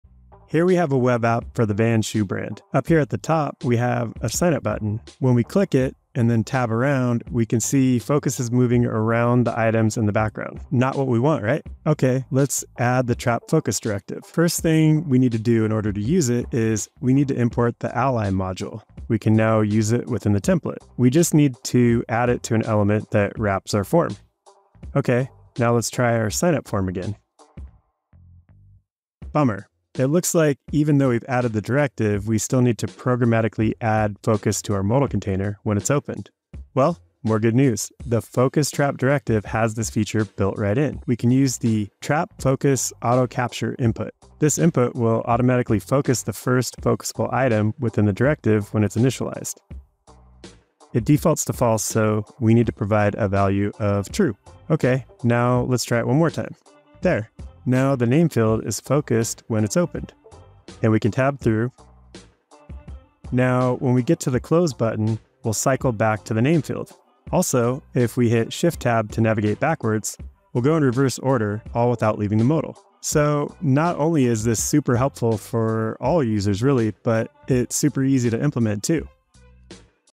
How to use the Angular CDK trap focus directive
A focus trap is an important accessibility feature. This video will help you understand what it is, why it’s important, and how to quickly add it in your Angular applications.